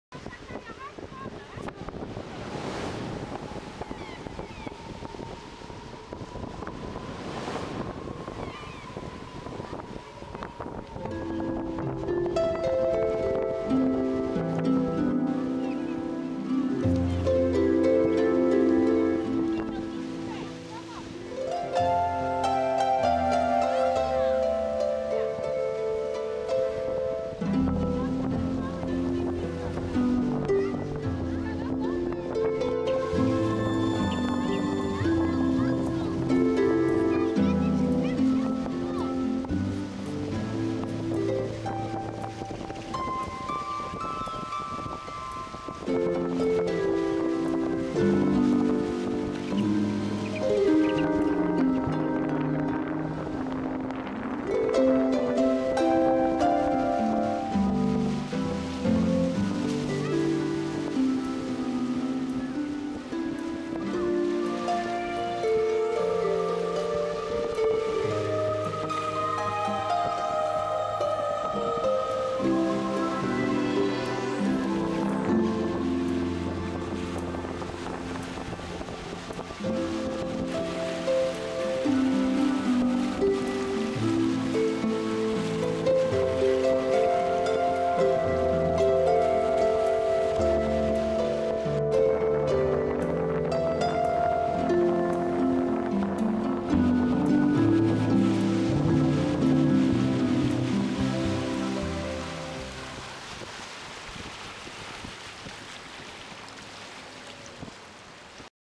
Steenlopers Katwijk aan Zee 23-02-2010
Steenlopers bij de uitwatering in Katwijk.